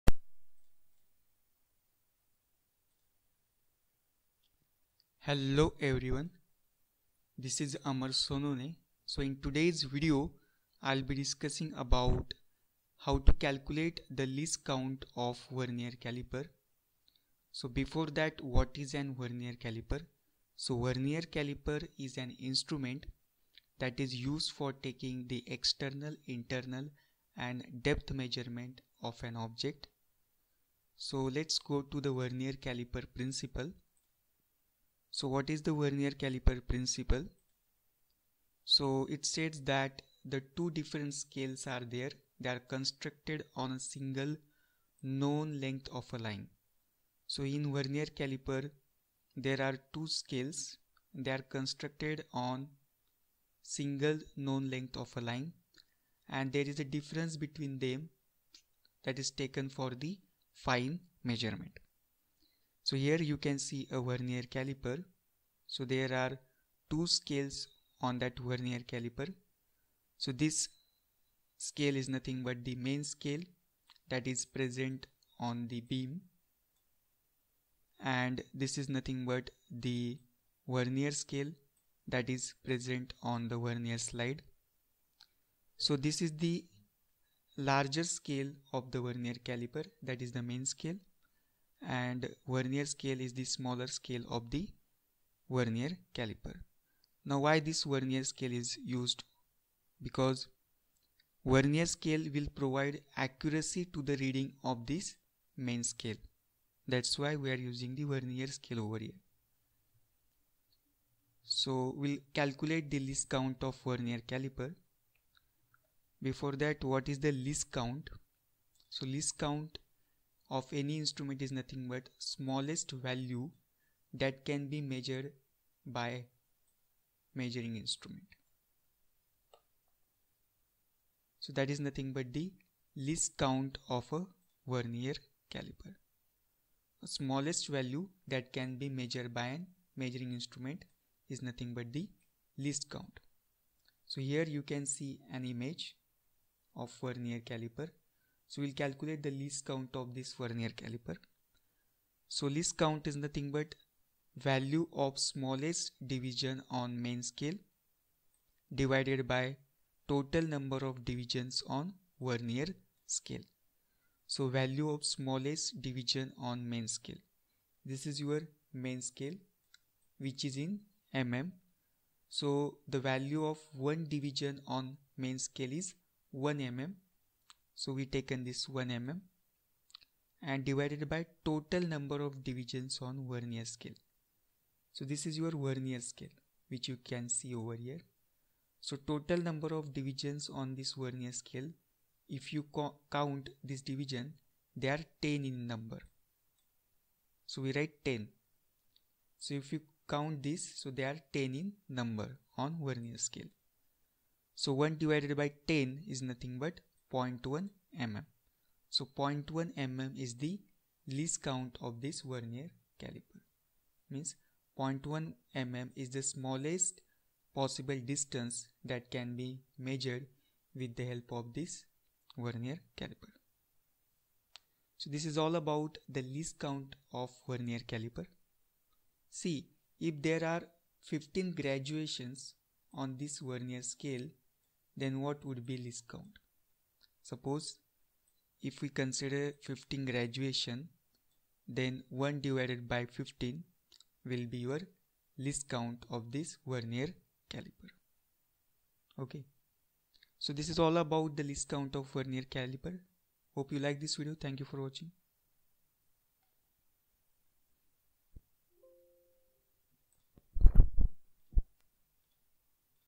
Least count of Vernier caliper
in this video i have covered about least count of vernier caliper